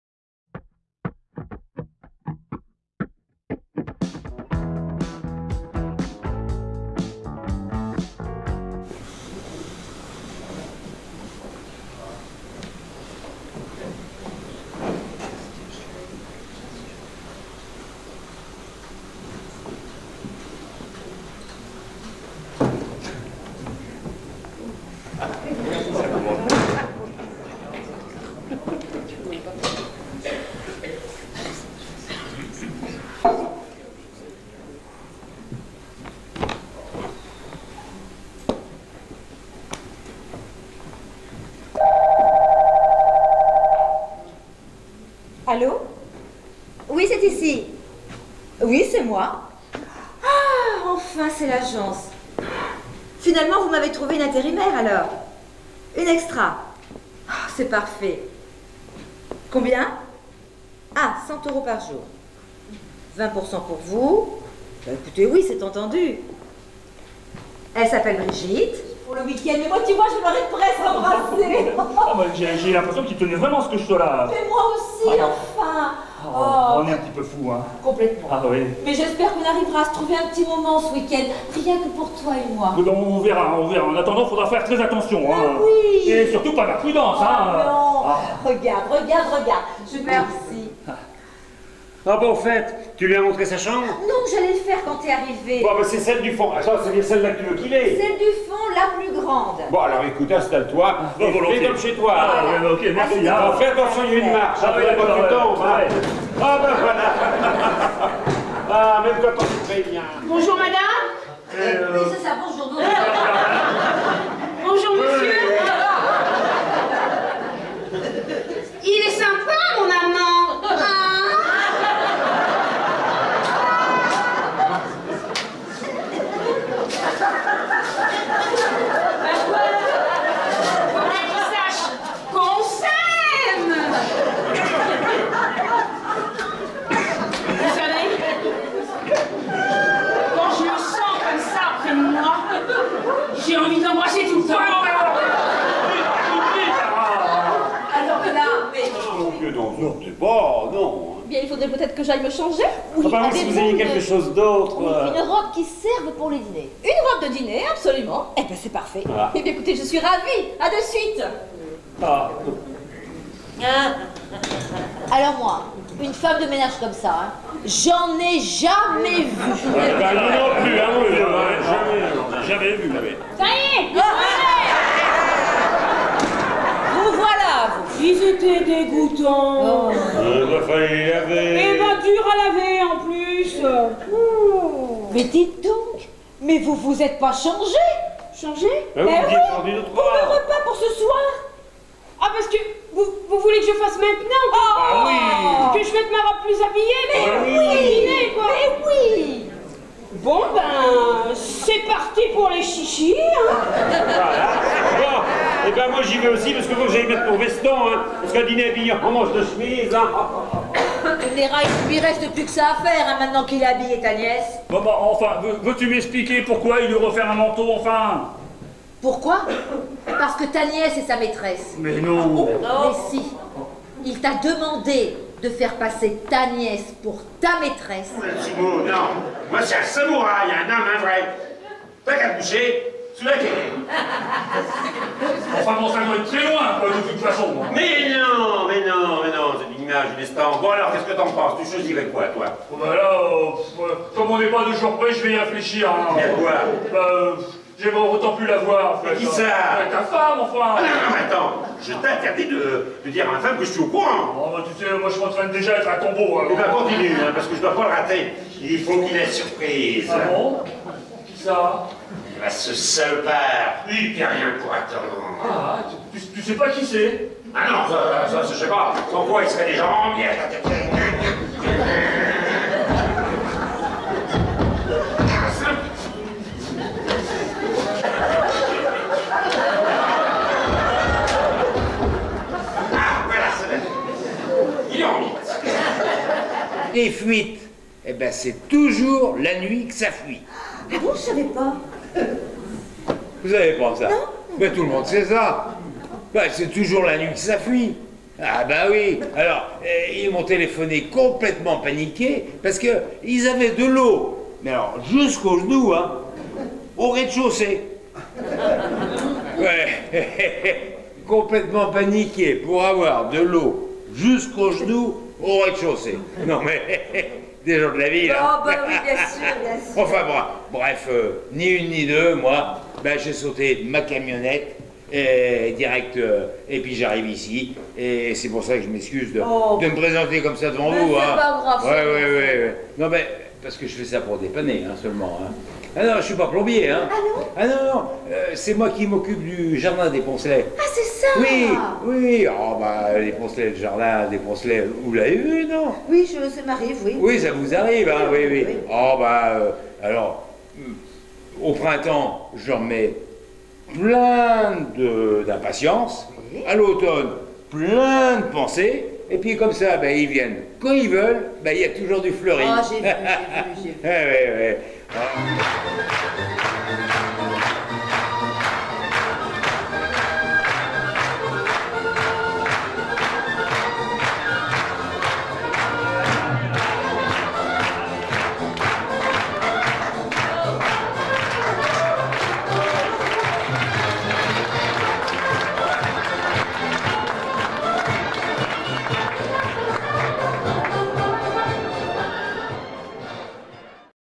THEATRE des BORDERIES * PYJAMA pour 6 * 2022
Fin de saison à SAINT JEAN D'ANGELY pour la troupe de théâtre des Borderies. Encore une soirée à guichet complet où on ne s'est pas ennuyé !
Une pièce à 100 à l'heure avec une floppée de rebondissements, de quiproquos,de mensonges en tout genre. Avec pyjama pour 6 le rire ne s'arrête jamais !!